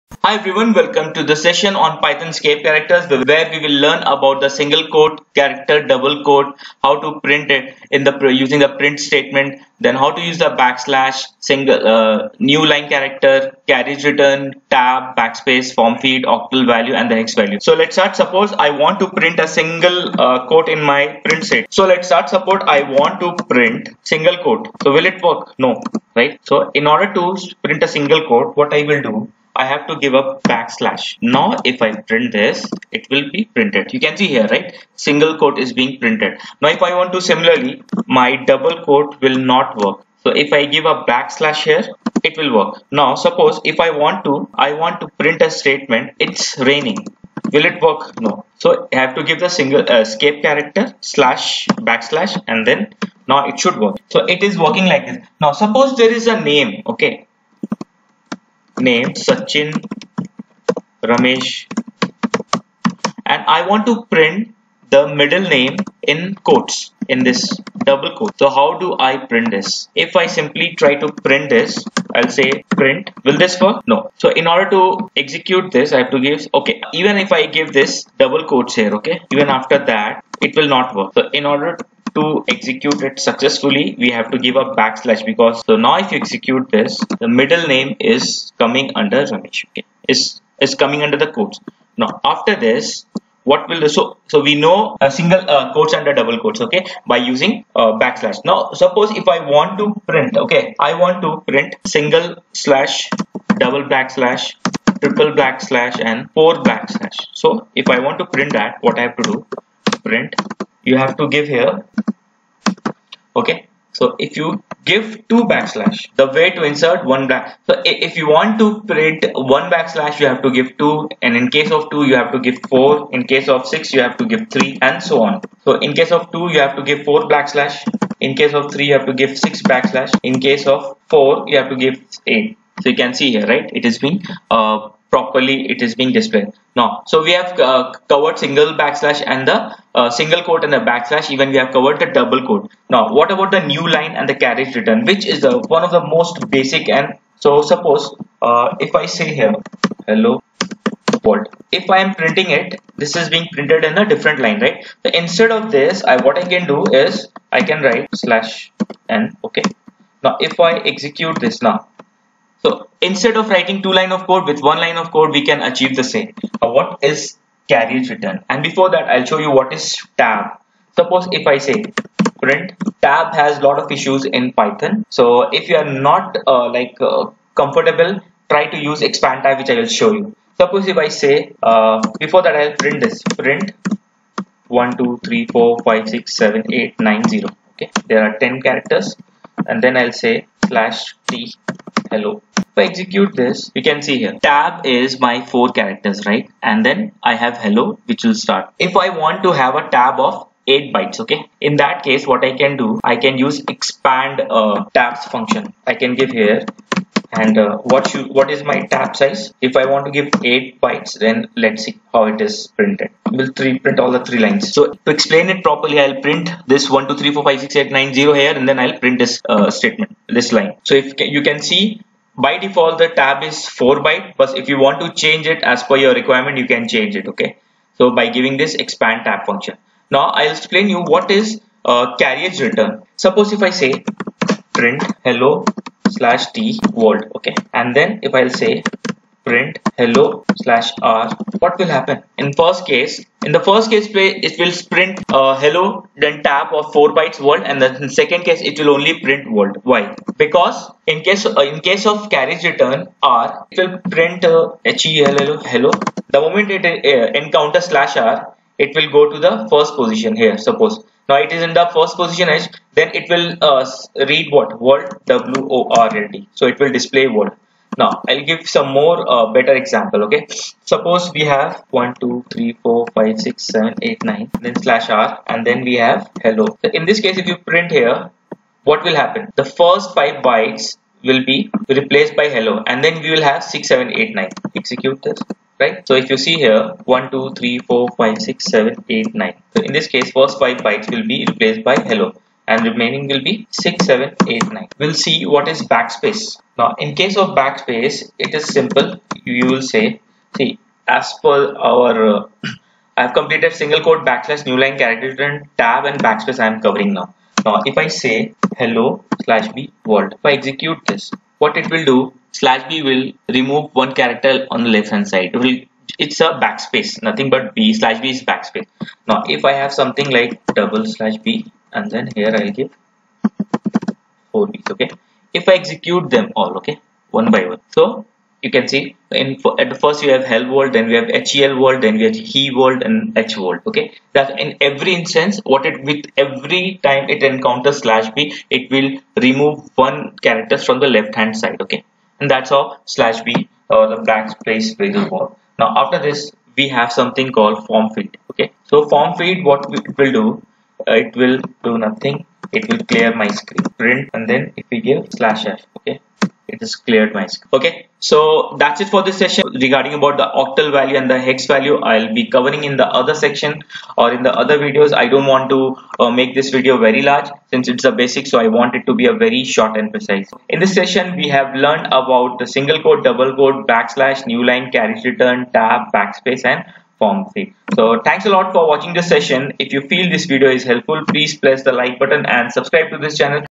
Python 3 Basics # 2.2 | Python Escape Characters | Escape Character in Python | Python for Beginners
Python Escape Characters
1. Single quote - Add Single Quote within a String
2. Double Quote - Add Double Quote within a String
3. BackSlash - How to insert backslash in a string
4. NewLine - How to insert a new line
5. Tab - How to use tab in python
6. Carriage Return - What is carriage retrurn and how to use it
7. Form Feed - What is Form feed
8. Octal Value - What is Octal Value
9. Hex Value - What is Hex Value

Code
print('\'')
print("\"")

print('It\'s Raining')

Sachin "Ramesh" Tendulkar

print("Sachin "Ramesh" Tendulkar")
print("Sachin \"Ramesh\" Tendulkar")

print('The way to insert one backslash is \\')
print('The way to insert two backslash is ')
print('The way to insert three backslash is ')
print('The way to insert four backslash is ')

print('Hello')
print('World')
print('Hello\nWorld')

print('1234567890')
print('\tHello')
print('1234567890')
print('\tHello'.expandtabs(tabsize=8))

print('Hello\tWorld')
print('Hello\rWorld')

Hello
World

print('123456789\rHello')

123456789
XXXXX
Hello6789

print('Hello\bWorld')
print('Hello\b\bWorld')
print('Hello\b\b\bWorld')
print('Hello\b\b\b\bWorld')

print('\f')